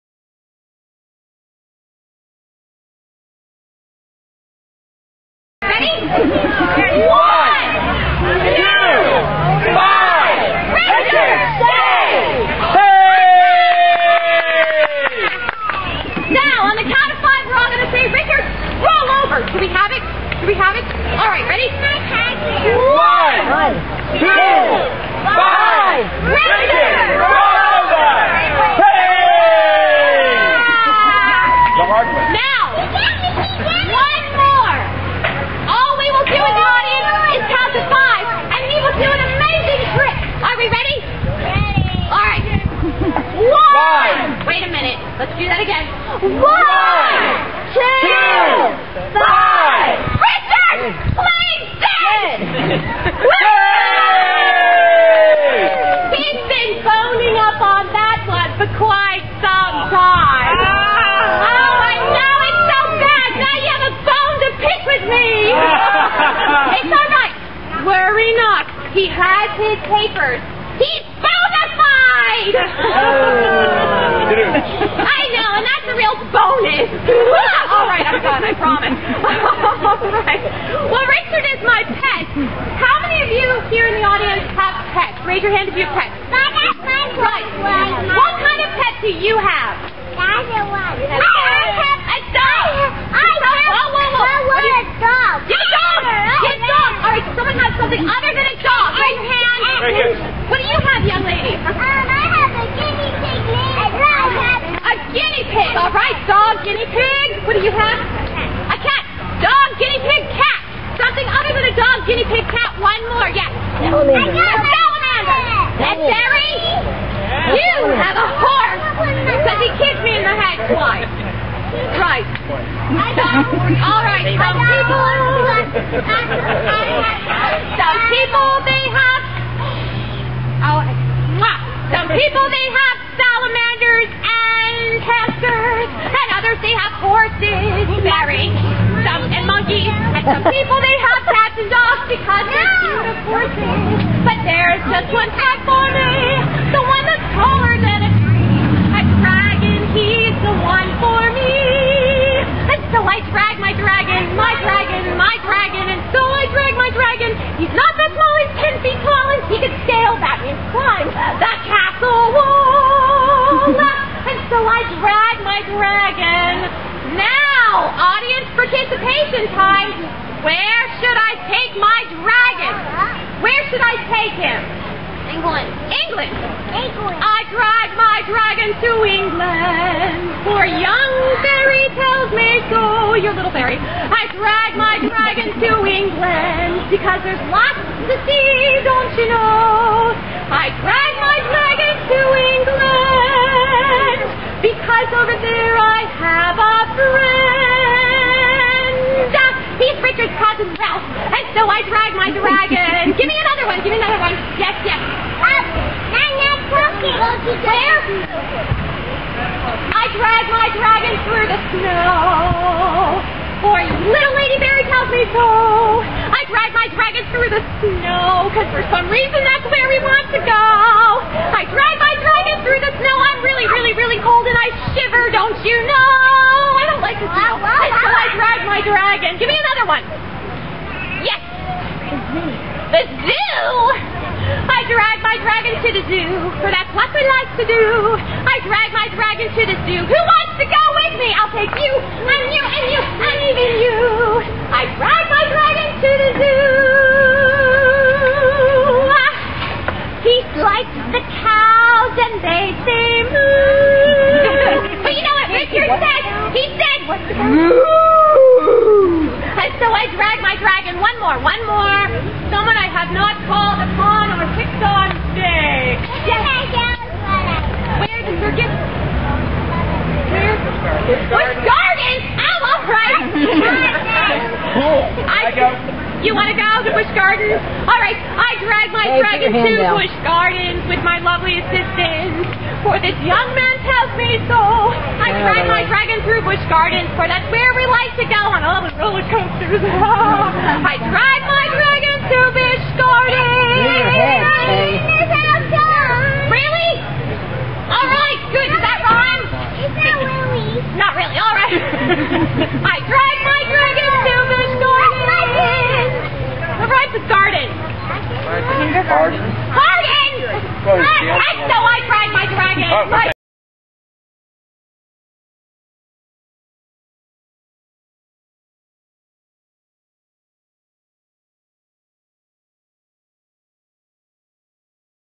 Mad Maggie, Gloucester Renaissance Fest, #1
Mad Maggie played by Melinda Griffith is a zany medieval character with a dragon bone pet that does tricks.  A must see show.  for more information about Mad Maggie.